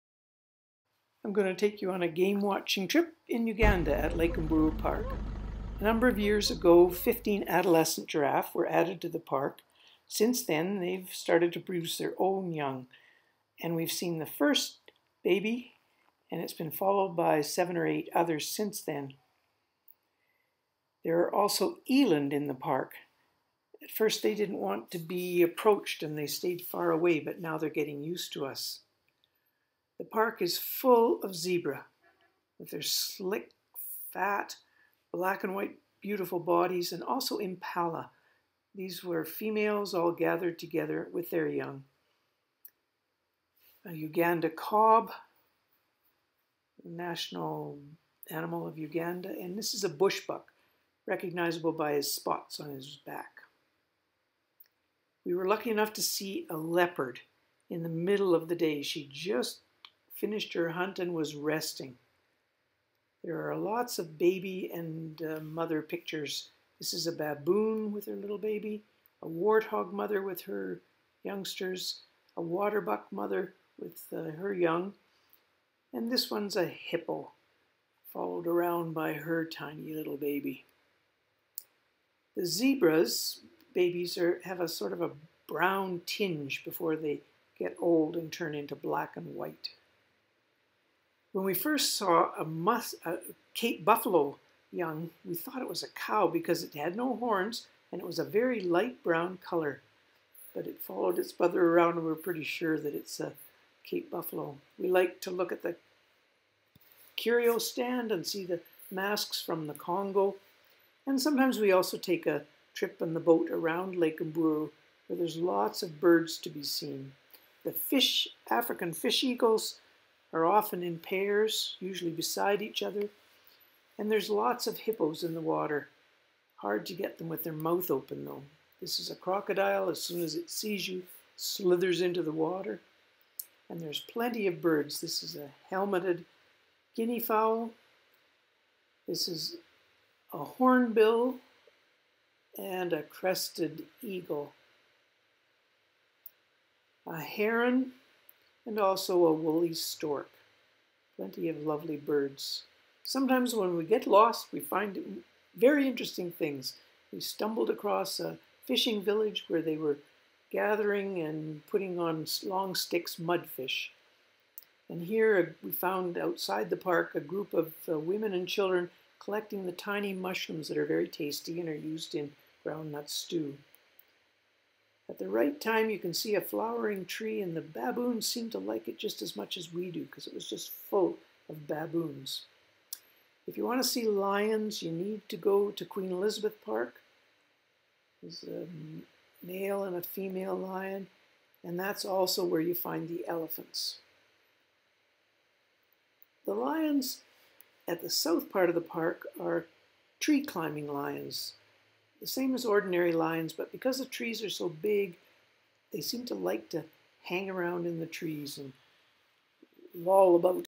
Game Watching at Lake Mburo National Park, Uganda
Photos from in and around Lake Mburo National Park, Uganda including giraffe, eland, leopard, zebra; various mothers and their young; some wonderful African birds with a few shots of elephants and lions from Queen Elizabeth National Park for my grand nephews and nieces.